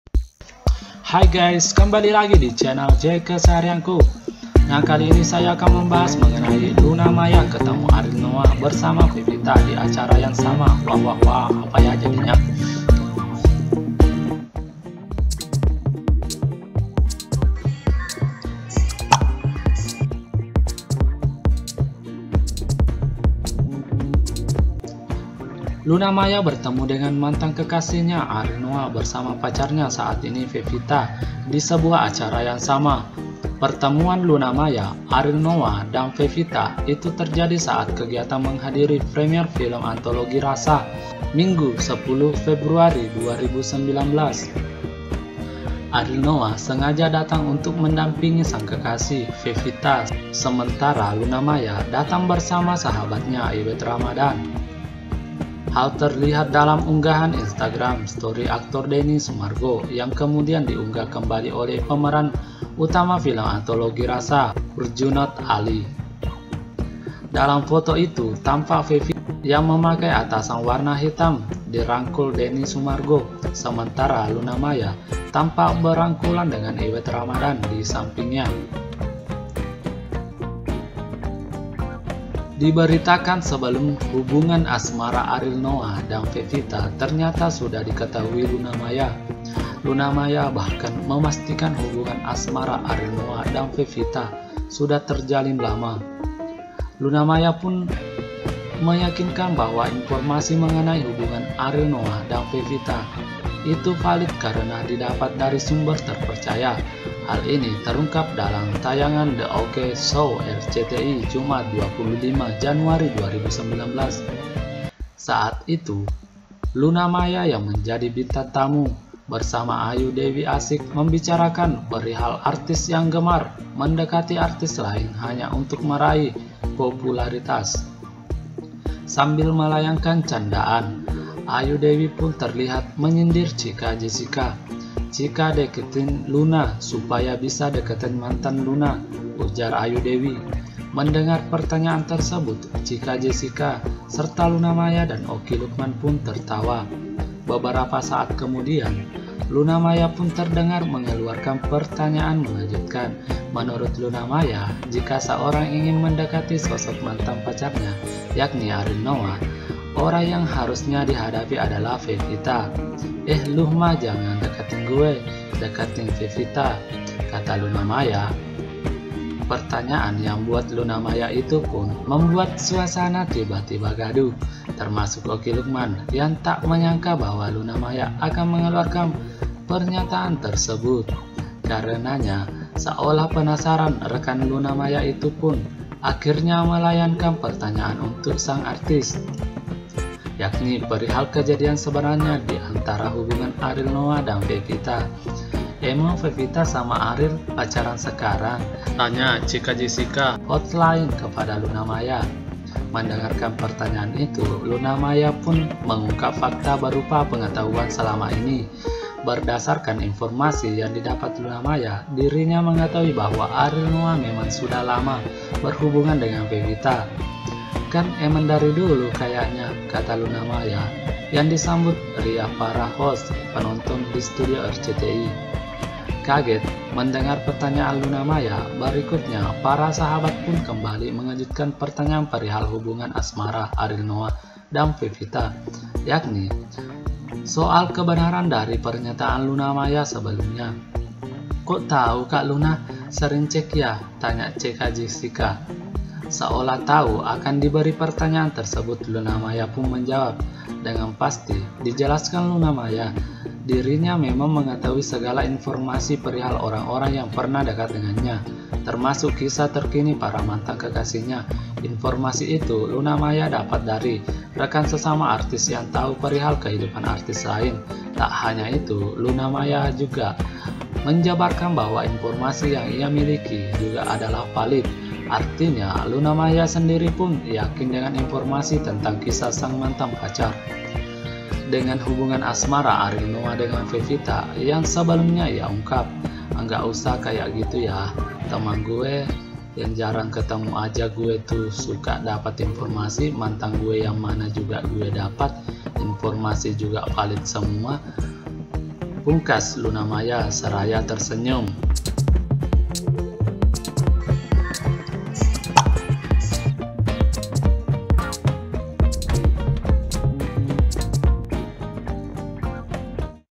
Luna Maya, Ariel Noah & Pevita bertemu di Acara yg sama
Ariel Noah, Luana Maya dan Pevita Dikabarkan bertemu  diacara yang sama, simak penjelasannyadalam video ini